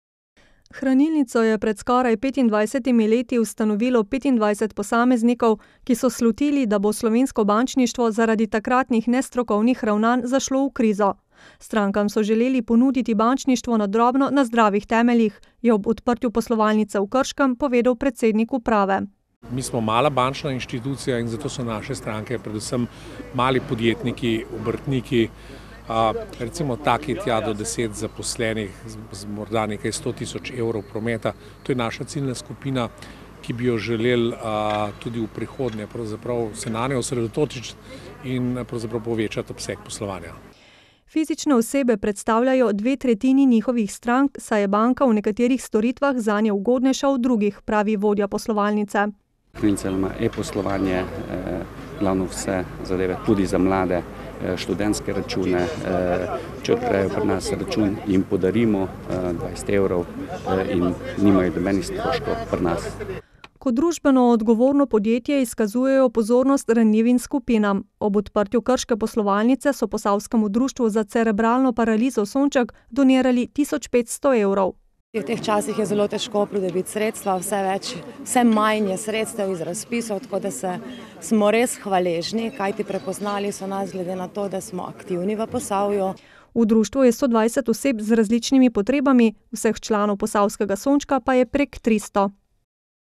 Odprli poslovalnico v Krškem
KRŠKO - Pred devetimi leti je v Brežicah odprla svojo poslovalnico Hranilnica LON. Zdaj pa so poslovno enoto odprli še v Krškem.